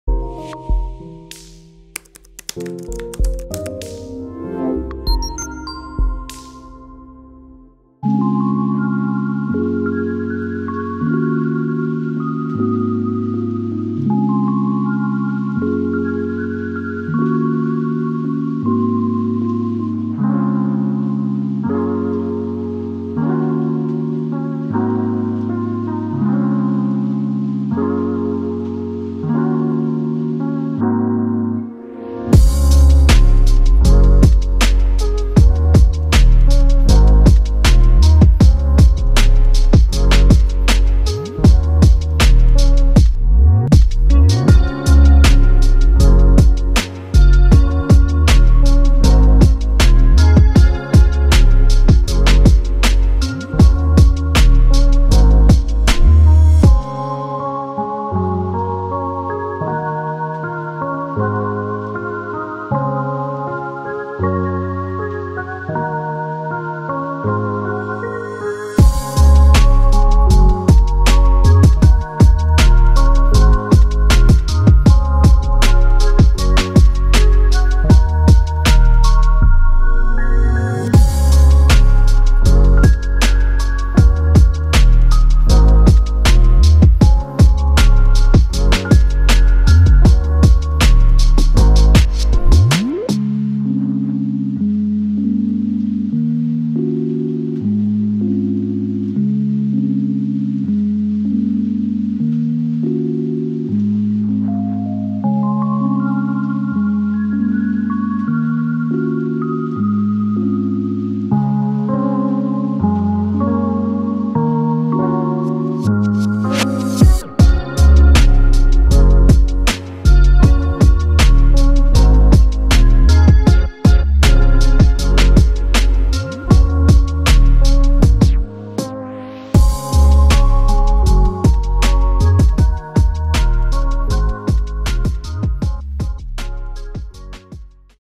2016 Swift Conqueror 565
Take a look around this 2016 Swift Conqueror 565, currently for sale at Highbridge Caravans. This is a 4 berth, single axle caravan with end washroom and fixed single beds. For price and further information see our online listing